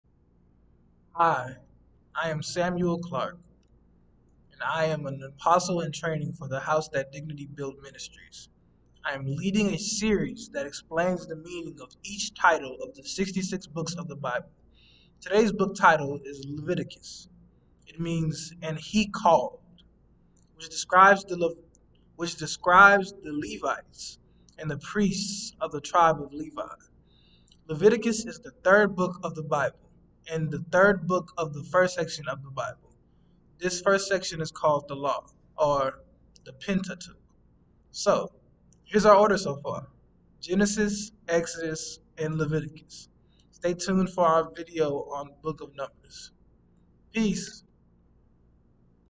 Dignity's Apostle-in-Training Series: Leviticus
Dr. Daintee Jones-Clark, Apostle of The House That Dignity Built Ministries, introduces Brother Samuel Clark, Apostle-in-Training, to present the meaning of the book title 'Leviticus'.